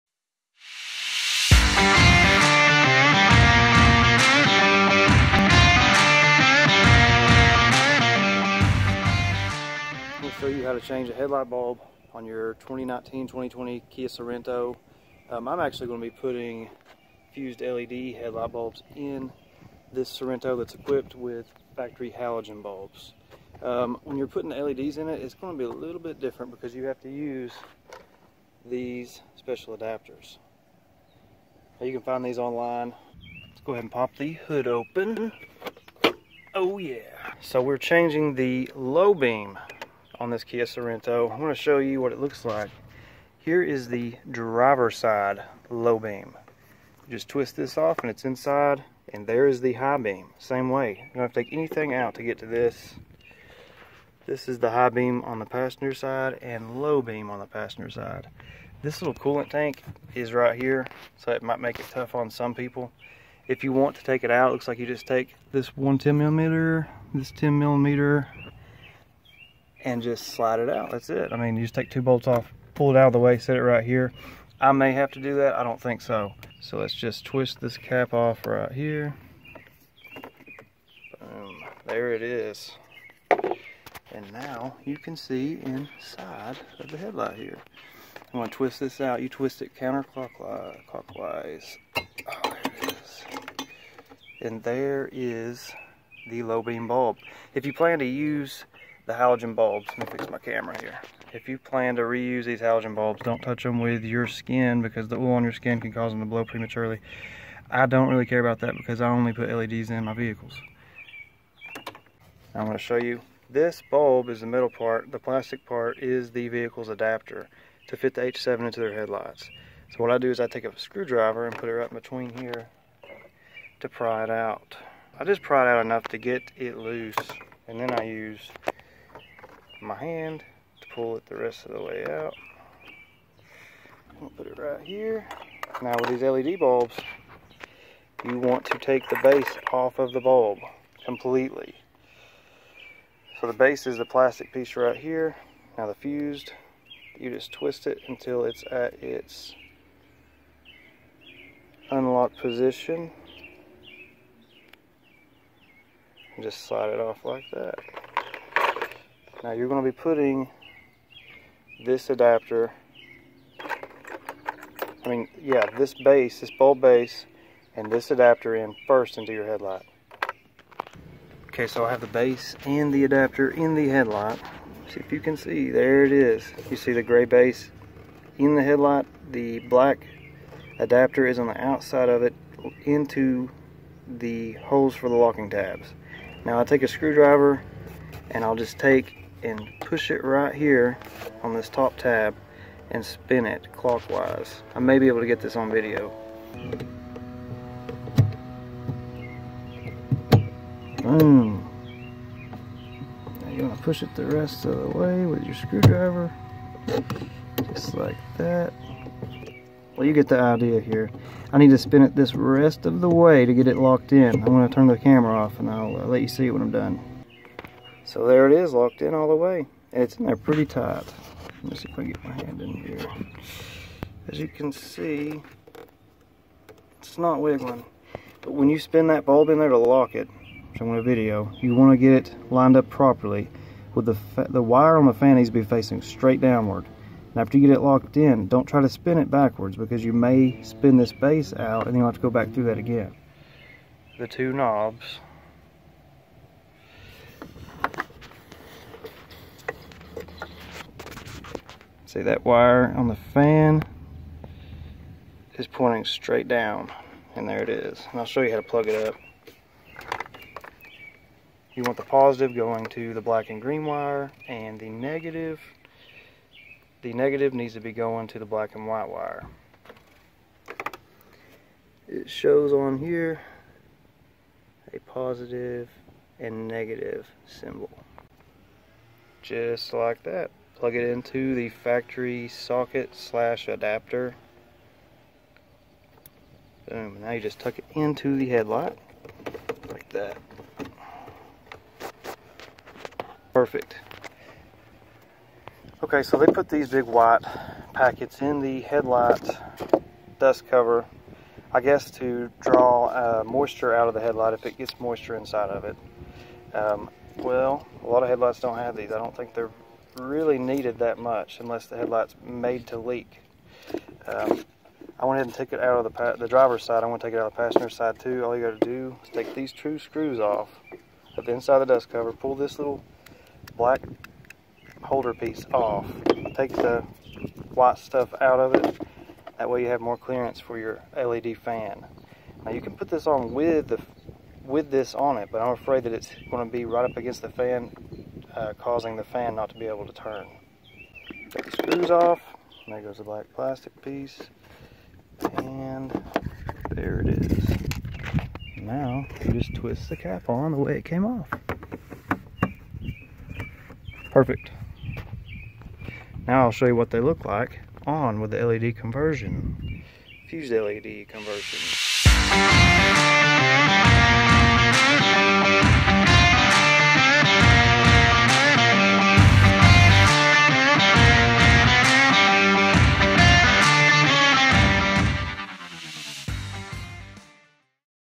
How to Replace Headlight Bulbs 2019-2020 Kia Sorento Install (+LED CONVERSION)
Low beam bulbs size: H7

LED Low Beam H7 Adapters

High beam bulb size: 9005

In this video I walk you through the replacement of your low beam headlight bulbs in your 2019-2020 Kia Sorento that is equipped with factory halogen headlight bulbs.

I decided to upgrade these with LED bulbs! I did not get to capture any night time driving shots. But believe me it was definitely a great upgrade in vision and appearance.